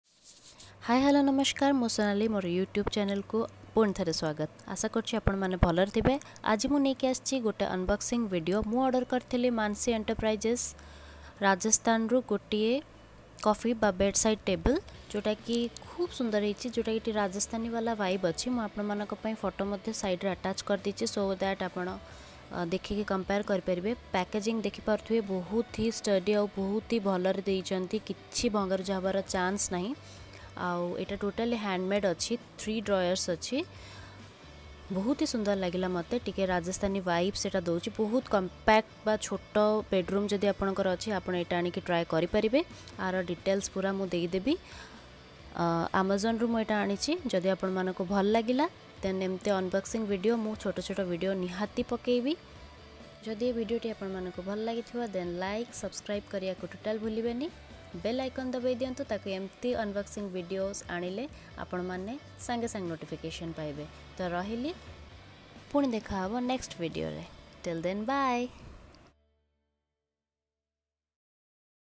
Amazon side Table | Unboxing
Amazon finds  modern wooden hand painted 3 drawers bedside office / coffee table for living room or bedroom Multicolour Rajasthan